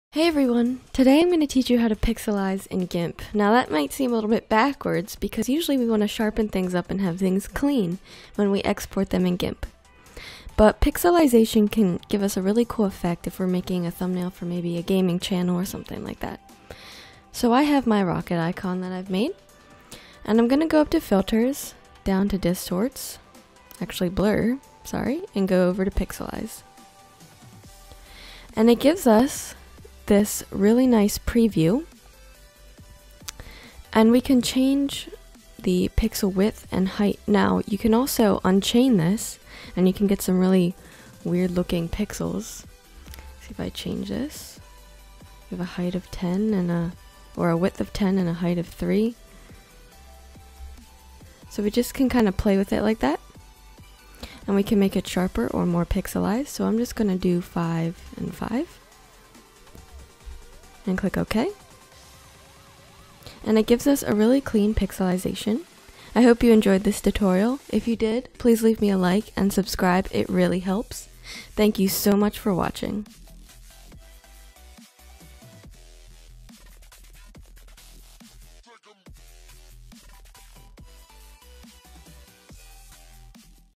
How to Pixelate Images in GIMP
Instead of sharpening up our images, today we are going to pixelate them. Learn how to use the Pixelize filter in gimp to create this cool effect for your projects.